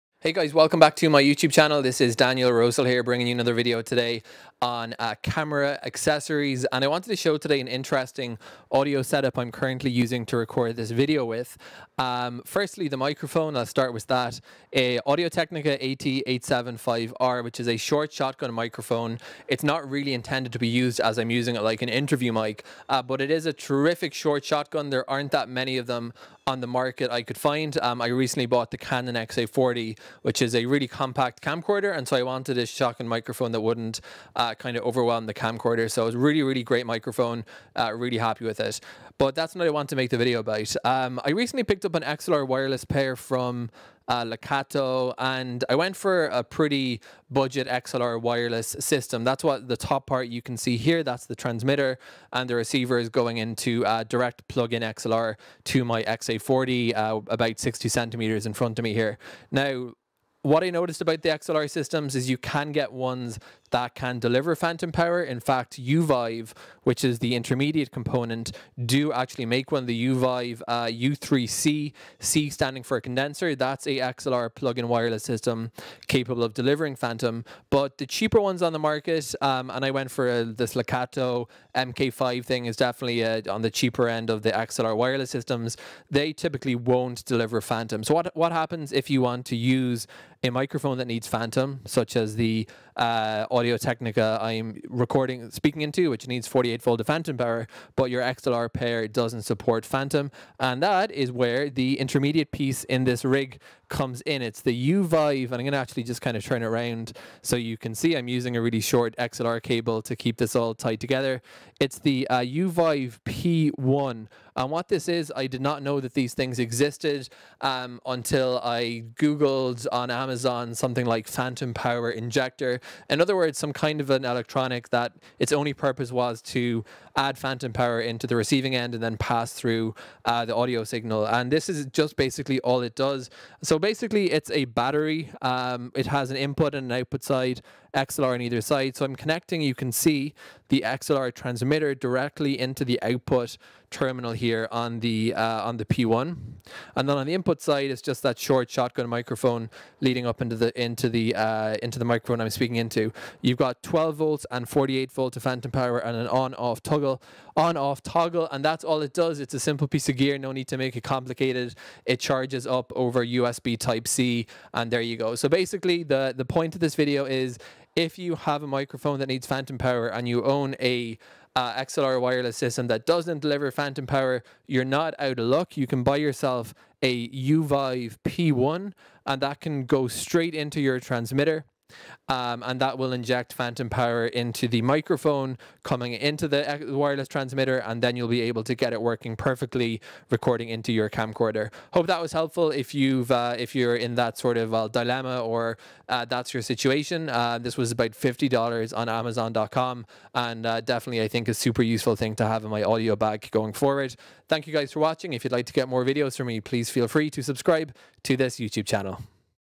Using Xvive P1 To Add Phantom Power To A Wireless XLR System
Using the Xvive P1 phantom power injector to add phantom power to a microphone and using it with a non-phantom equipped XLR wireless pair.